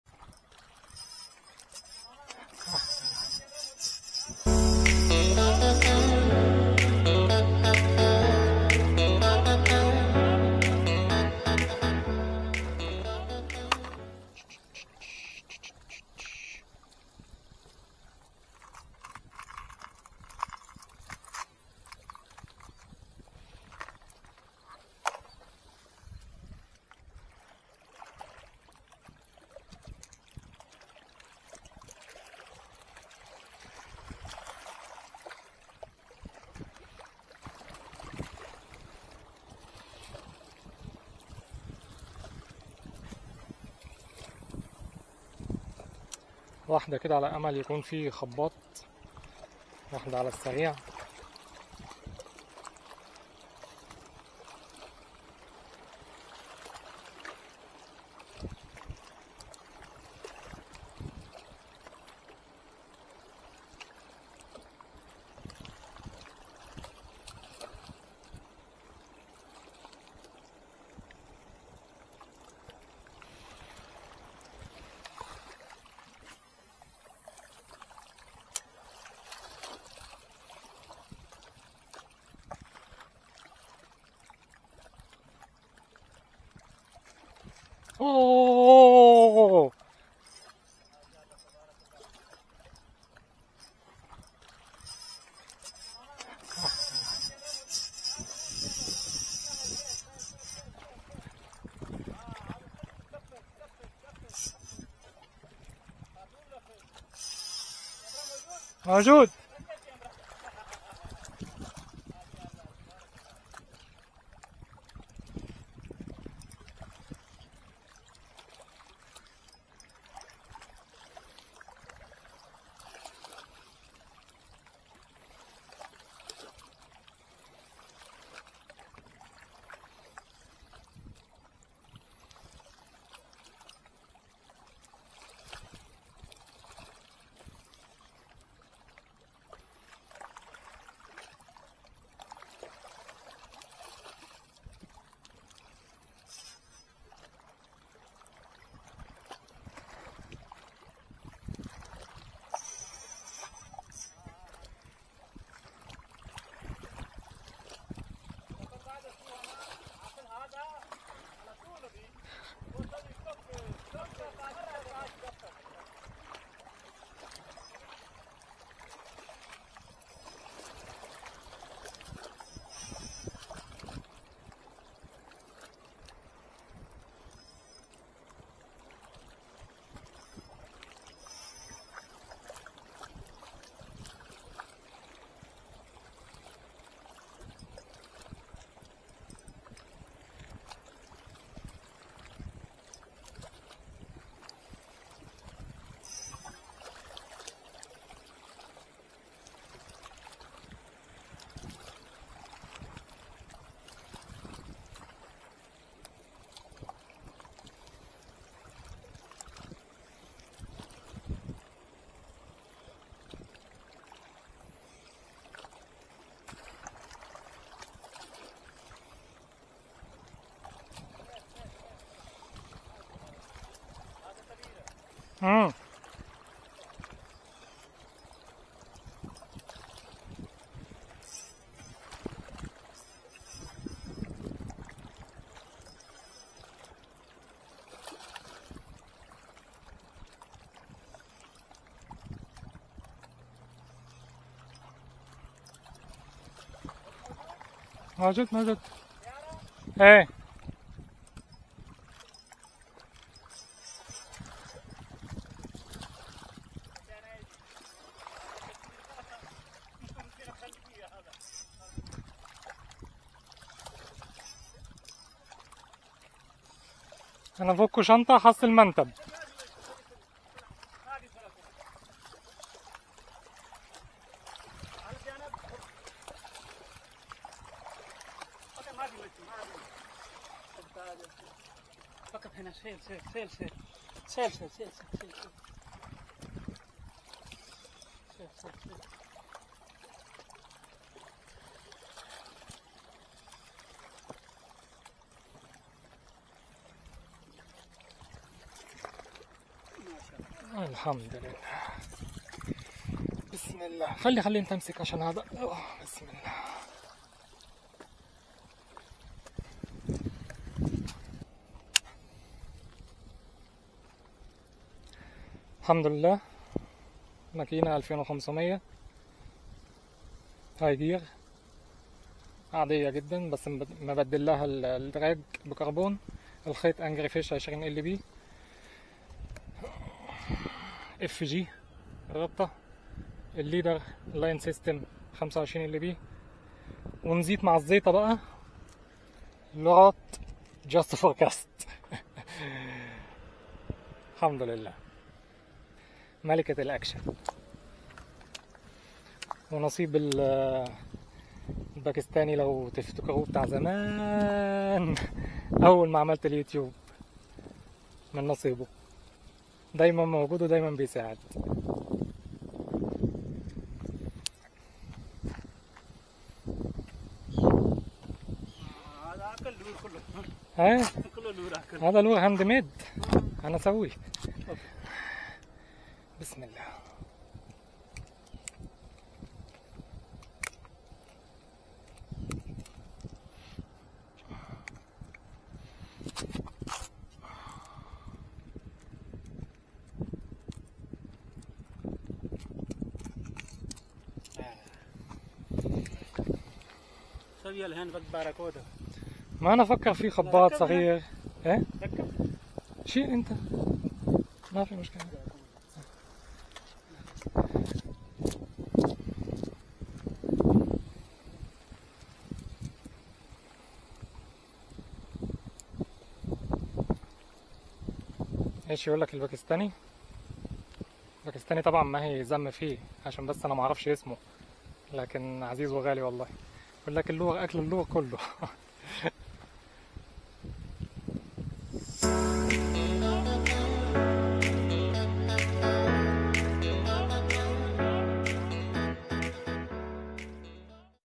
صيد السمك كاستنج واكشن حلو قبل نهاية اليوم / good action with the queen
صيد السمك كاستنج  واكشن جميل قبل نهاية اليوم

✅I'm on Instagram as @mohamedsallem1. Install the app to follow my photos and videos.
➡️Rod\ lineaeffe / 2.1m / 5~20g.

➡️Reel\  Chinese  / 2500 size.

➡️Line\ Angryfish X9 / 20 lp.